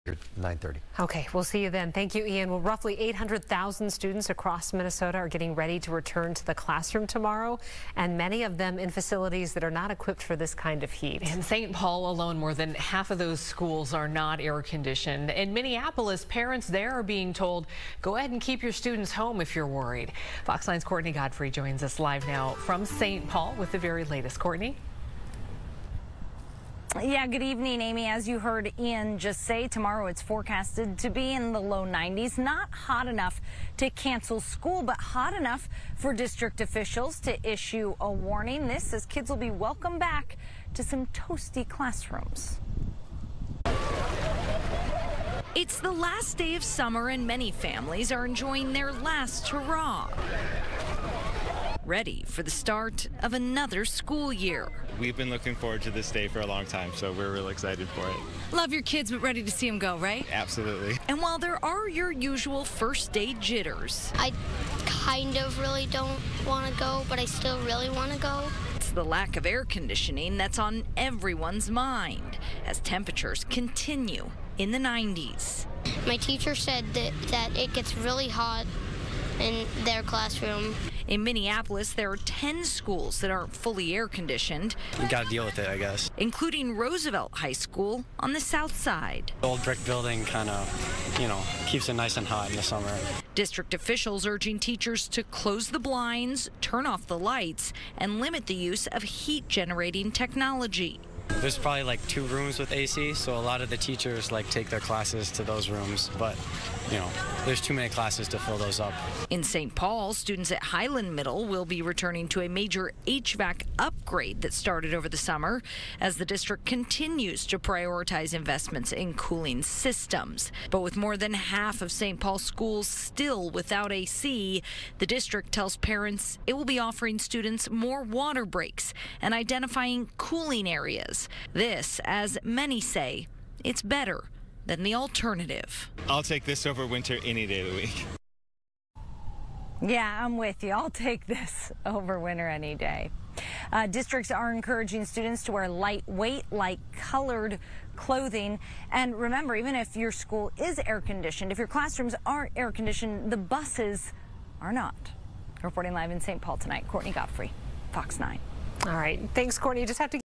Students face sweaty start to school year
For students in Minnesota who return to class on Tuesday, it will be a hot and sweaty start to the school year.